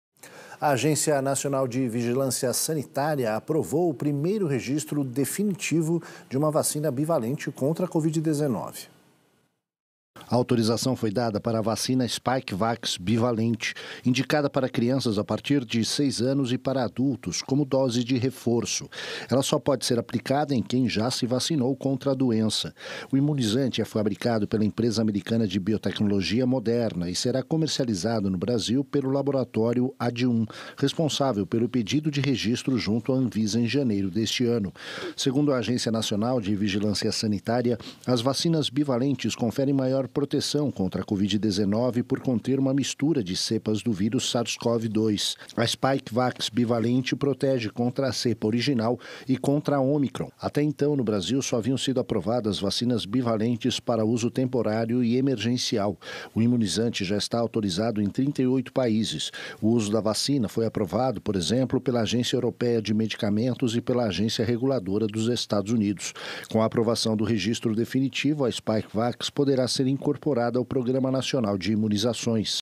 Anvisa aprova primeiro registro definitivo à vacina bivalente contra Covid-19
A autorização foi dada para a vacina Spikevax Bivalente, indicada para crianças a partir de 6 anos e para adultos como dose de reforço. Ela só pode ser aplicada em quem já se vacinou contra a doença.

O imunizante é fabricado pela empresa americana de biotecnologia Moderna e será comercializado no Brasil pelo laboratório Adium, responsável pelo pedido de registro junto à Anvisa, em janeiro deste ano.

Segundo a Agência Nacional de Vigilância Sanitária, as vacinas bivalentes conferem maior proteção contra a Covid-19, por conter uma mistura de cepas do vírus SARS-CoV-2.